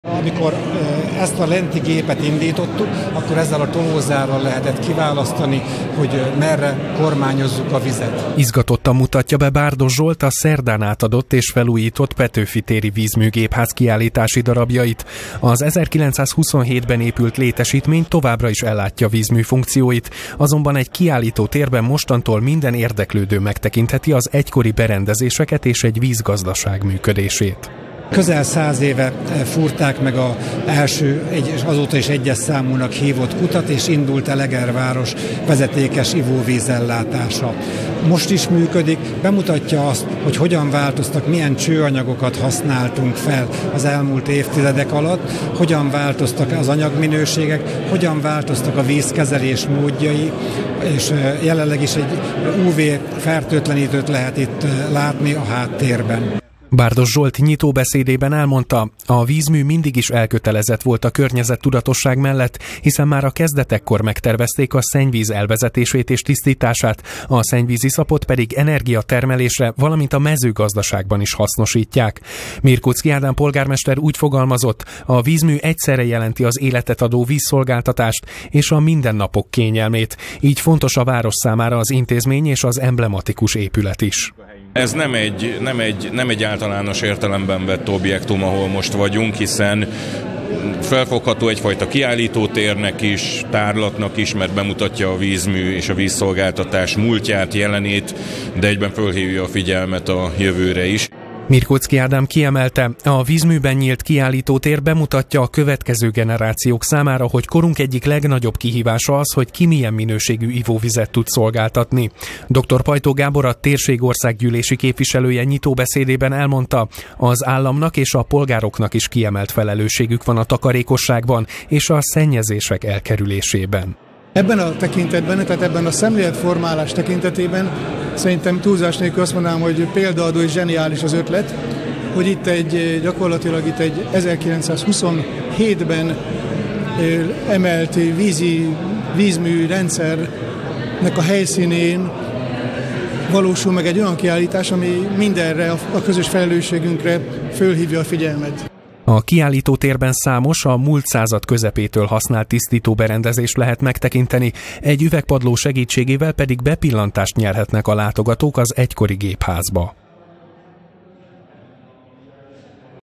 Átadták a felújított Petőfi téri vízműgépházat - 2023.04.26.
Átadták a felújított Petőfi téri vízműgépházat szerdán délelőtt. Az épületben mostantól egy tárlat is megtekinthető, mely az egykoron használt tisztítóberendezéseket mutatja be. A nyitóbeszédekben elhangzott: az 1927-ben épült ingatlan szimbolikus jelentőséggel is bír, hiszen reprezentálja a felelős vízgazdálkodás és a környezetvédelem fontosságát.